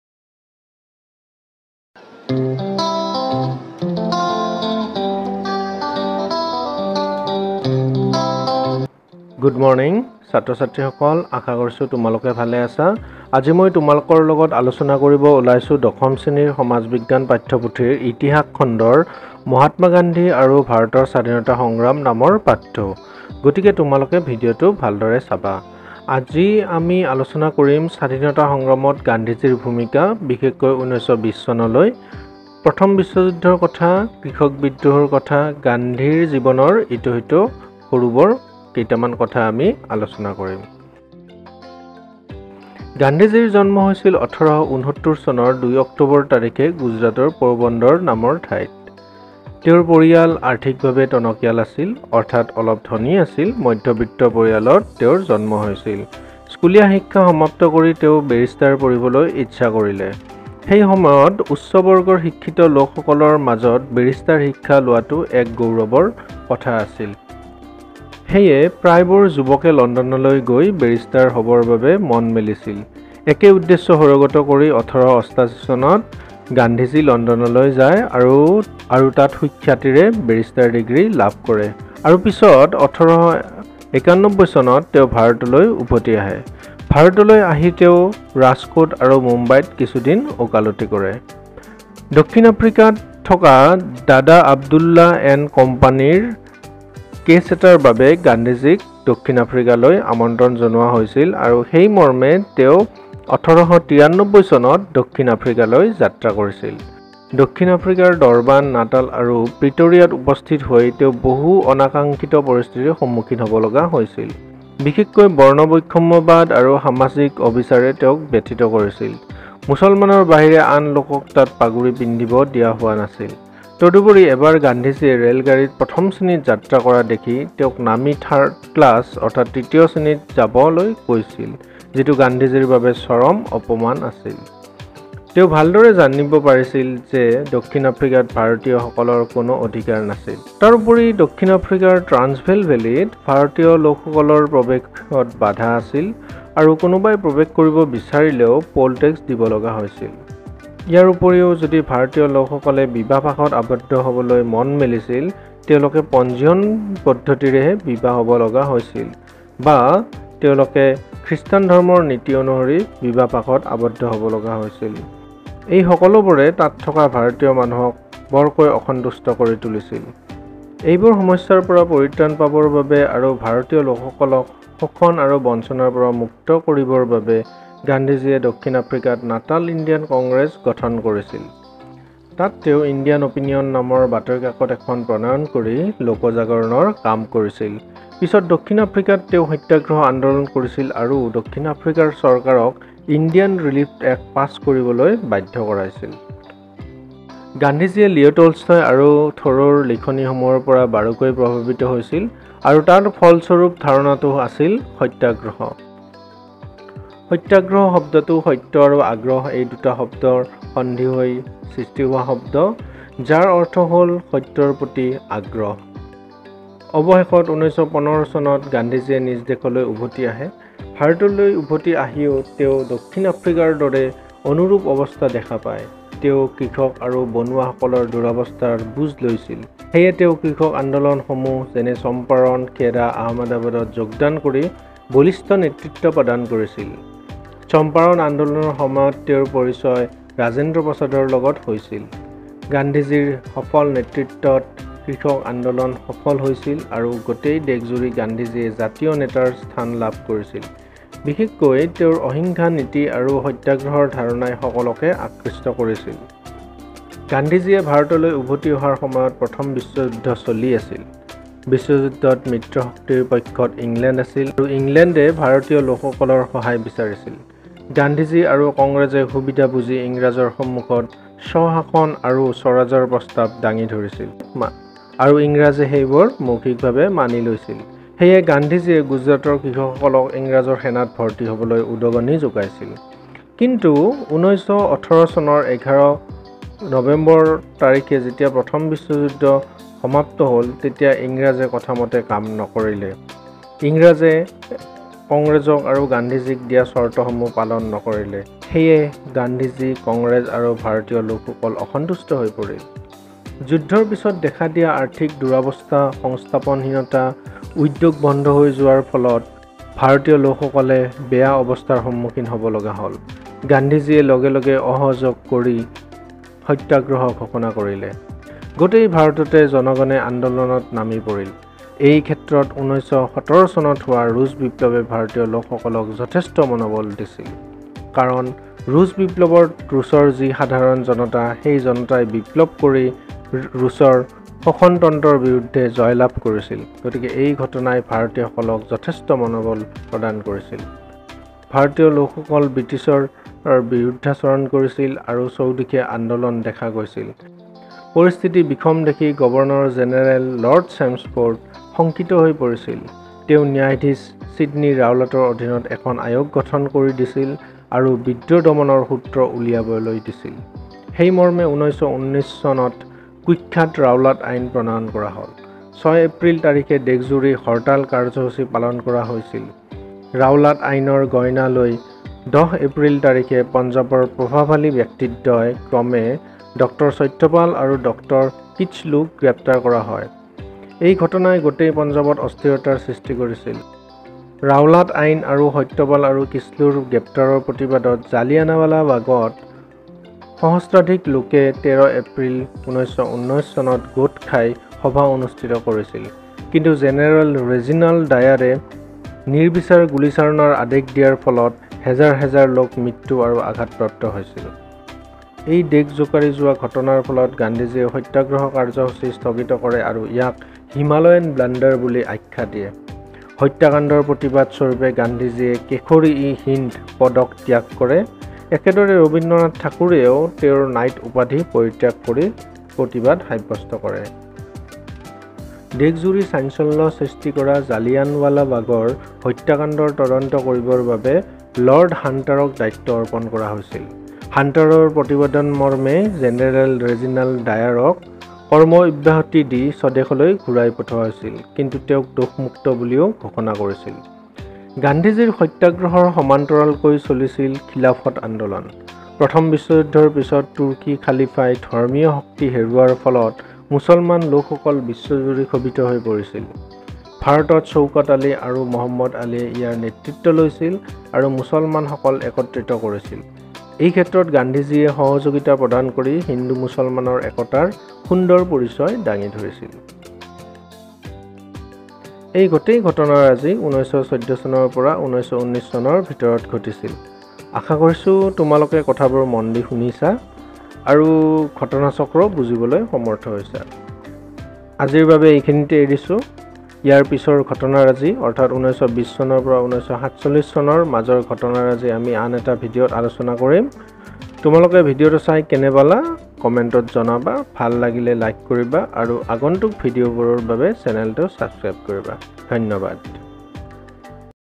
Class 10 Social Science: History: Mahatma Gandhi and Indian Freedom Movement Explained In Assamese
Mahatma Gandhi played an active role in Freedom movment. Coming back from South Africa he engaged himself in various Farmer rebelions eg. Champaran, kheda and Ahmedabad rebelion and brought justice by means of Satyagraha.

SCERT ASSAM Class 10 social science offers a brief history of freedom movement of India. In this video a brief account of freedom movement is given right from 1888 to 1919.

Language of the Video: Assamese.